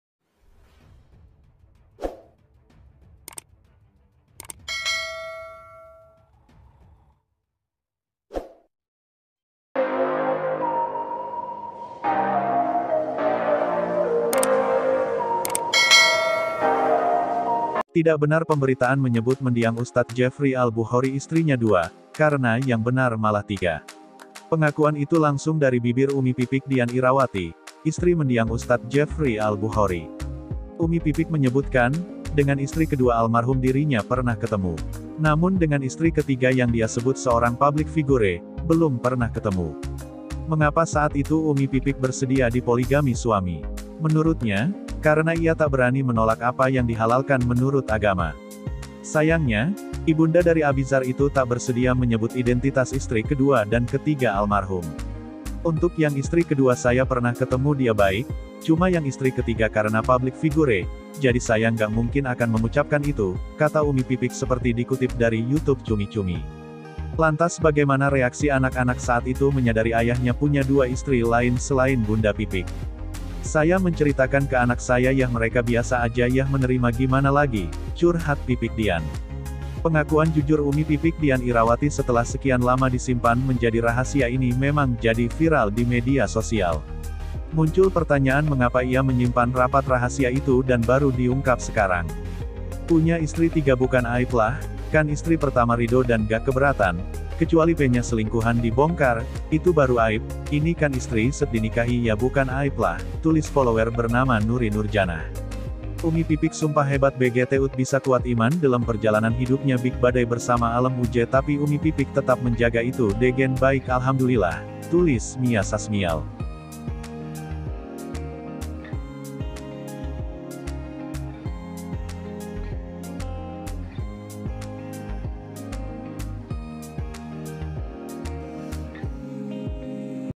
Tidak Benar Mendiang Ustad Jefri Istrinya 2 Yang Benar Itu 3 Umi Pipik Istri Ke-3 Public Figure
Tidak benar pemberitaan menyebut mendiang Ustadz Jefri Al Buchori istrinya 2, karena yang benar malah 3.Pengakuan itu langsung dari bibir Umi Pipik Dian Irawati, istri mendiang Ustadz Jefri Al Buchori.